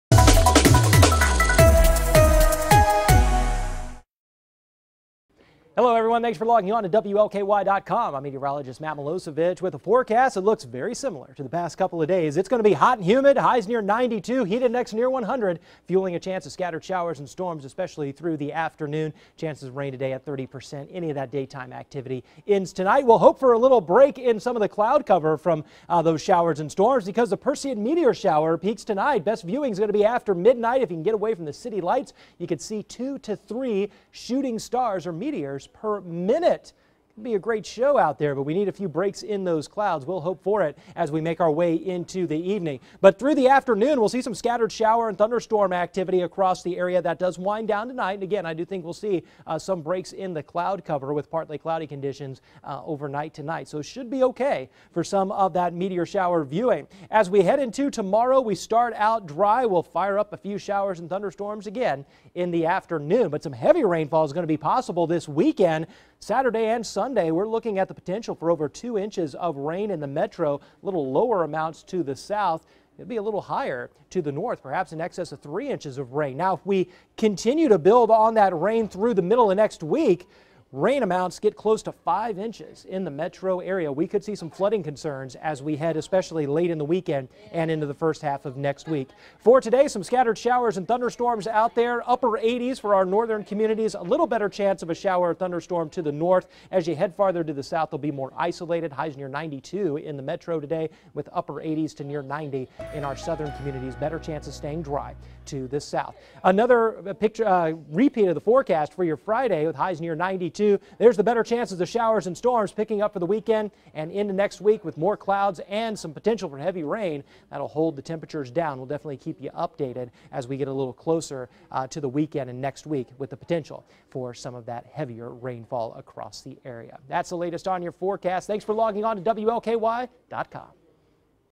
Dodging showers and staying hot
Updated 9:00 AM ET: WLKY's Matt Milosevich has the latest Thursday forecast.